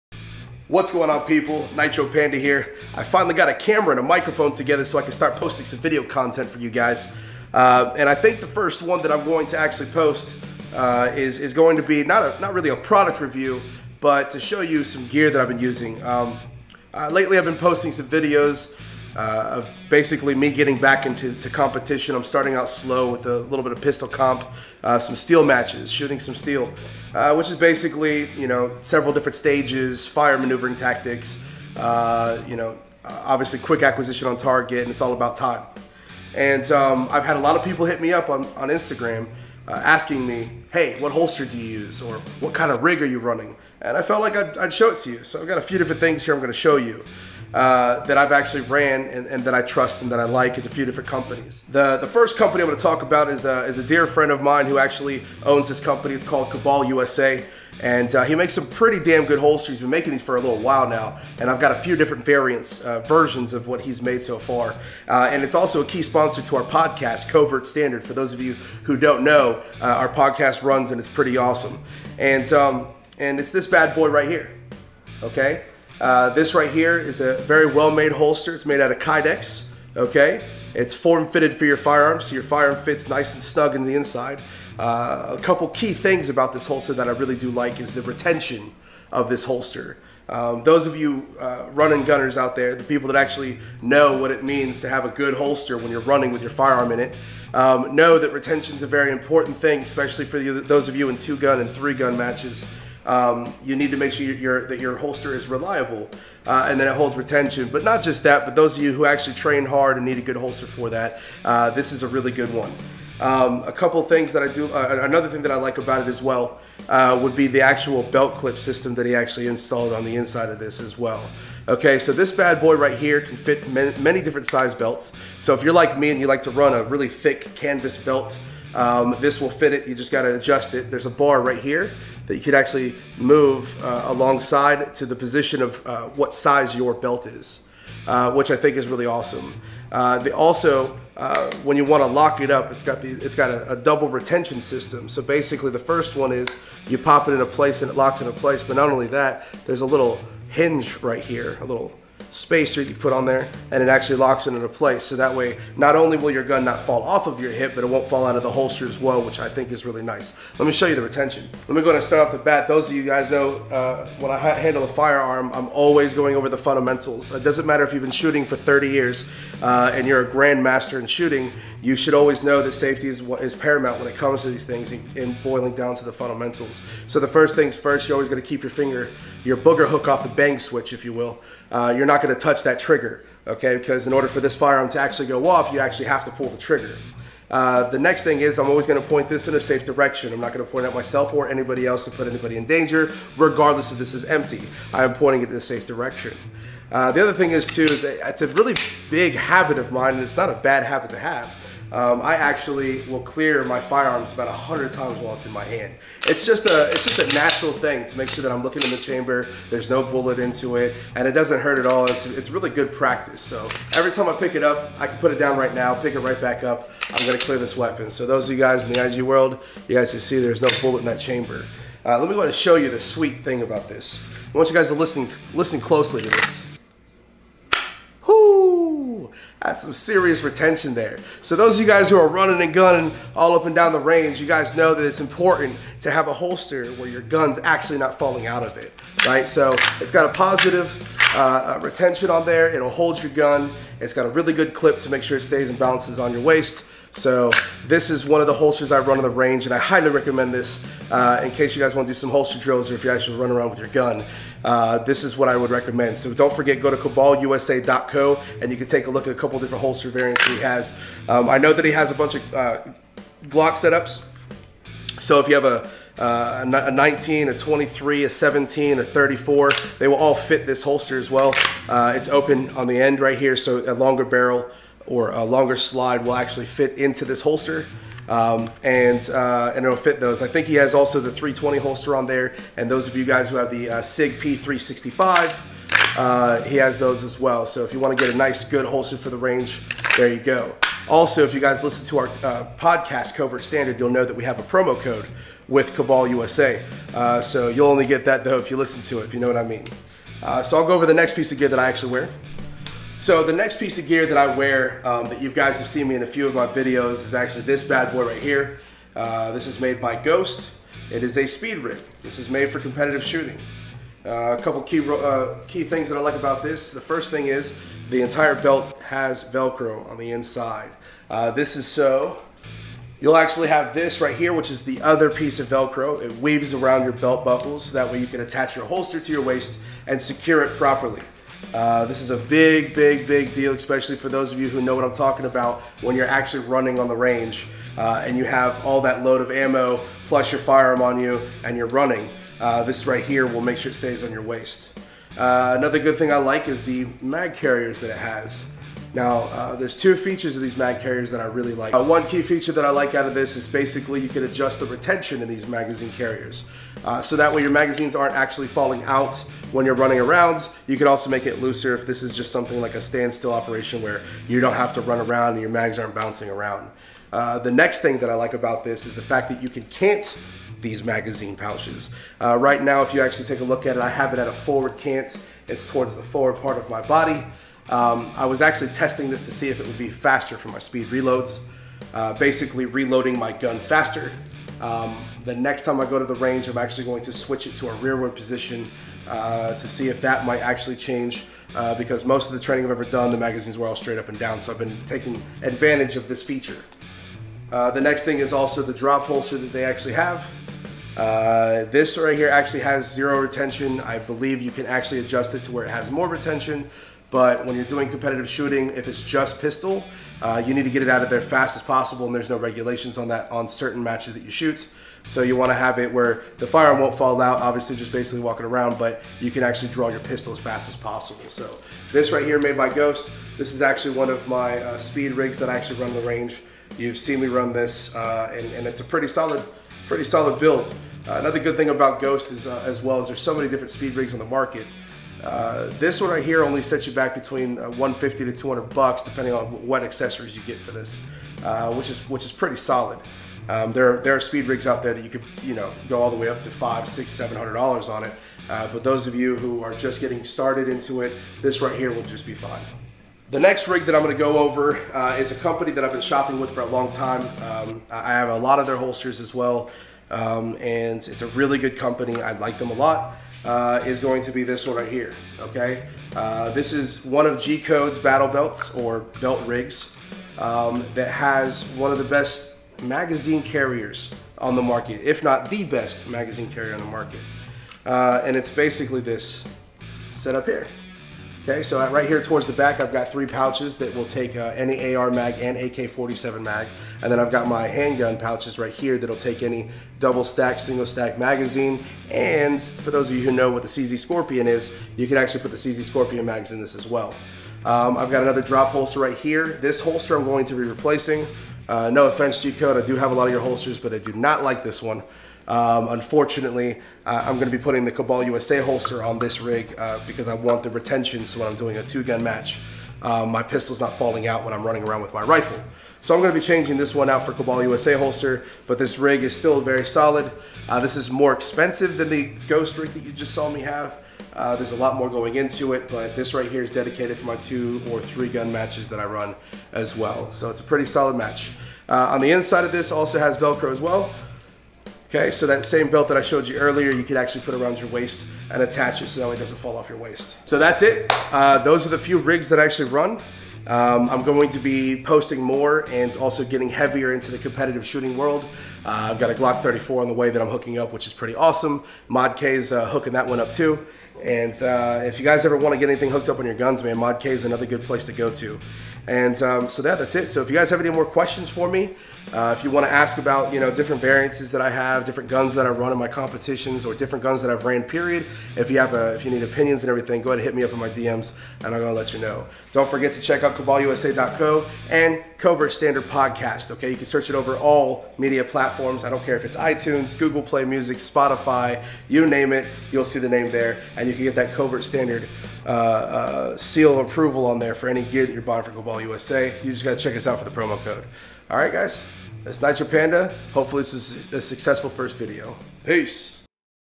Gear Check!!
There have been a lot of people asking me what gear I use on the range so here it is!!